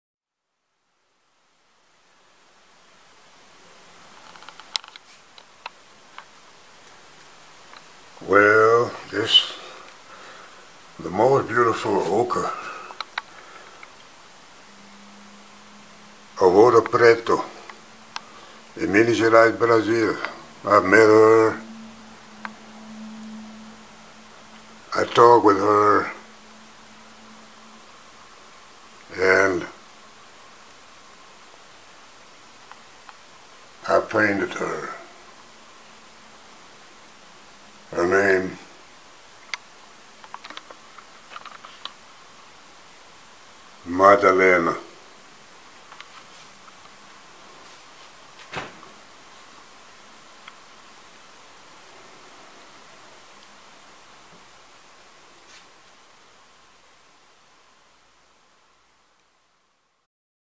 MADALENA the sweetest hooker of 'black gold' , Brazil {}}{}{ painting 2007
© RamblinW\art - oil on board
''í met her in a small historical town of ''ouro preto'', black gold where back then the portugues robbed this metal and killed the Indians. Madalena was a whore, had two children and her profession was to survive... in this place of many churches build by the slaves in order of the Portugues rulers or tyrants...'